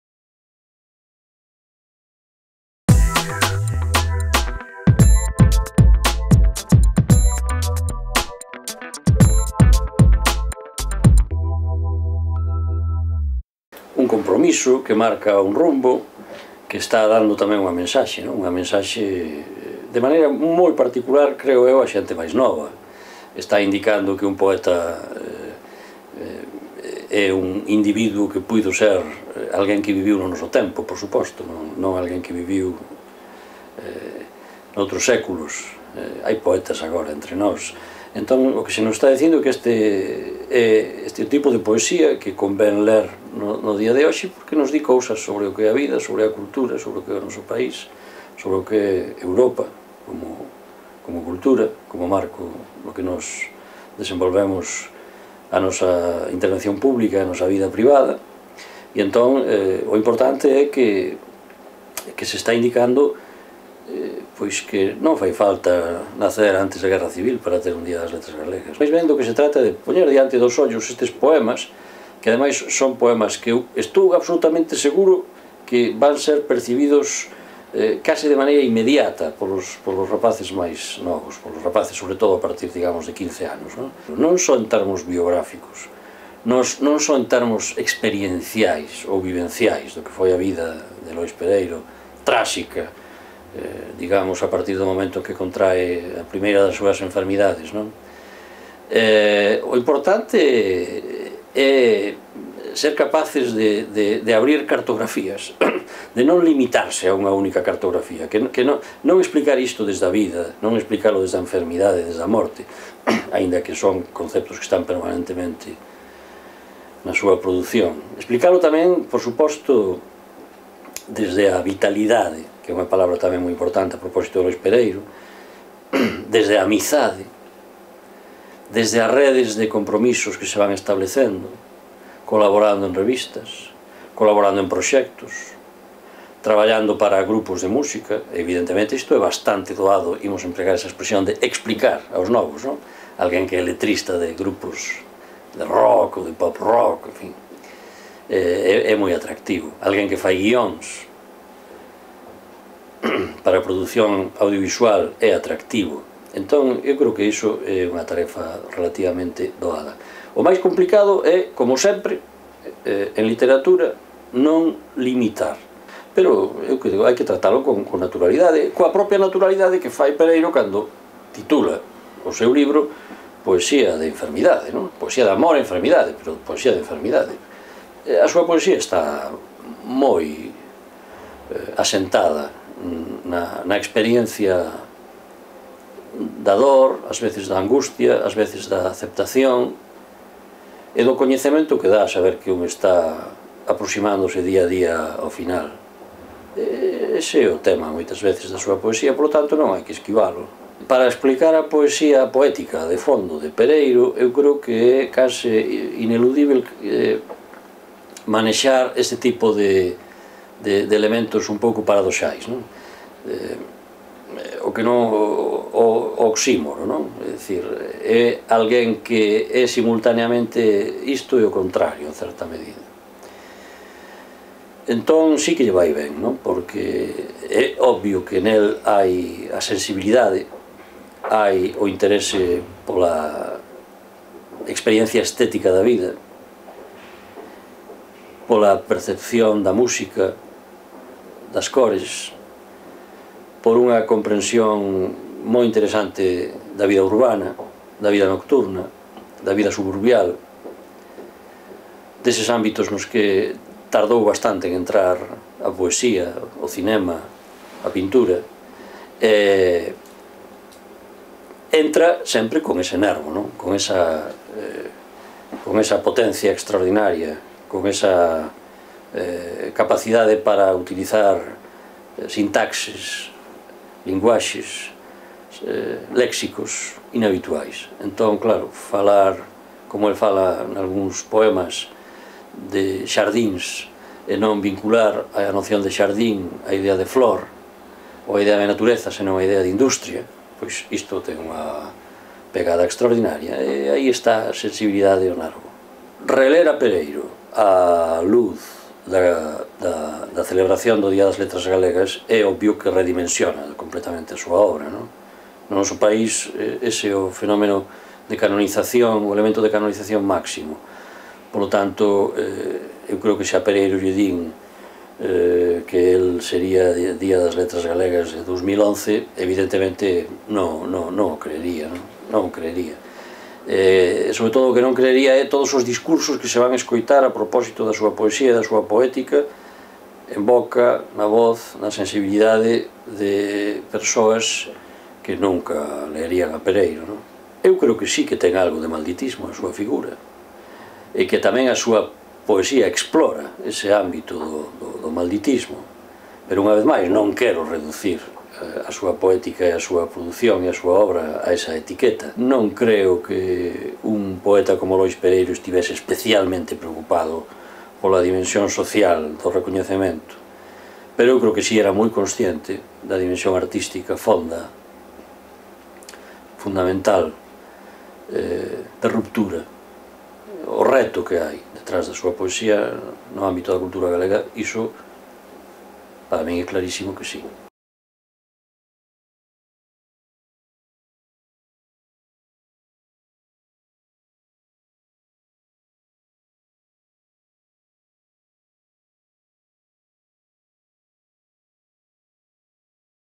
Unha cartografía de Lois Pereiro
Arturo Casas analiza a poética de Lois Pereiro